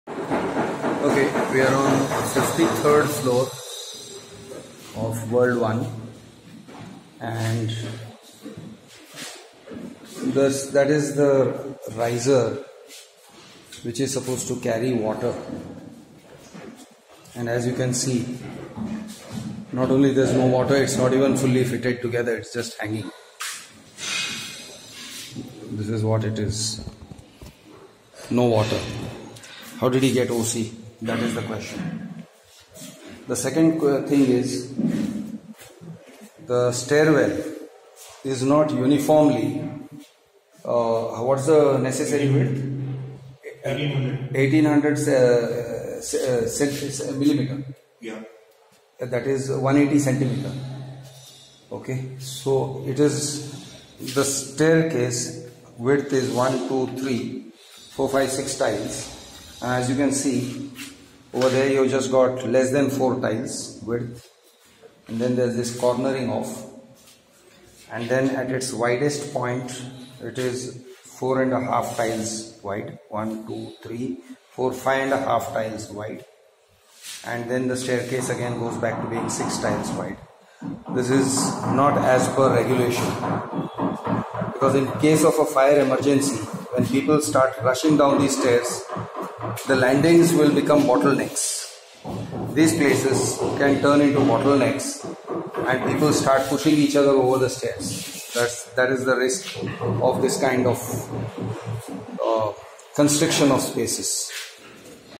Lodha World One: 53rd floor stairwell -- firefighting water supply incomplete, but they got OC?
Raw video from our recent recce of Lodha World One, World Towers, Lower Parel, Mumbai. The project is developed by Shreeniwas Cotton Mills, a company taken over by Lodha Developers, now renamed as Macrotech Developers. It shows that the water supply of the fire-fighting system was still unconnected. How did this floor get a Partial Occupation Certificate?